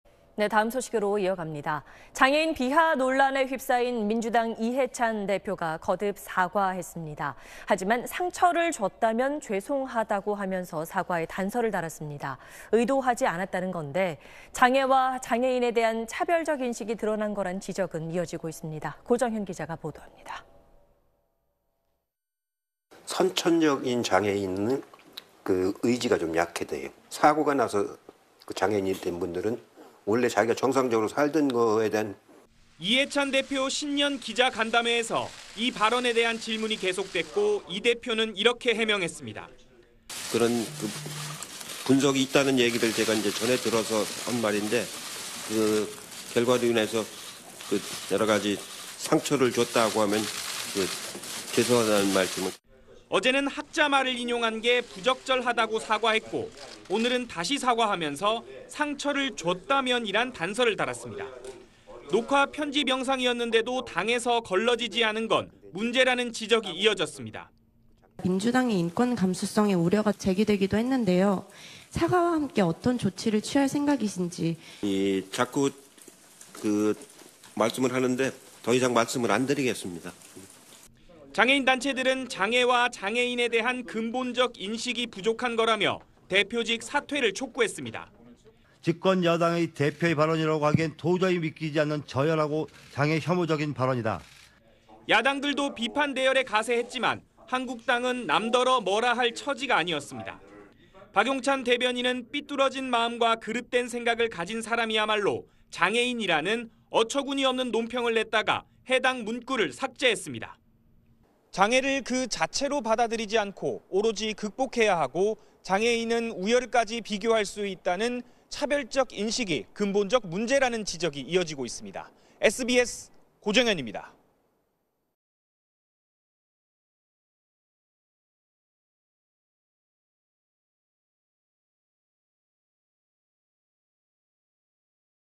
이해찬 "상처 줬다면 죄송"…장애인 차별적 인식이 문제 / SBS
장애인 비하 논란에 휩싸인 민주당 이해찬 대표가 거듭 사과했습니다. 하지만 '상처를 줬다면 죄송하다고' 하면서 사과에 단서를 달았습니다. 의도하지 않았다는 건데 장애와 장애인에 대한 차별적 인식이 드러난 거라는 지적은 이어지고 있습니다.
고정현 기자가 보도합니다.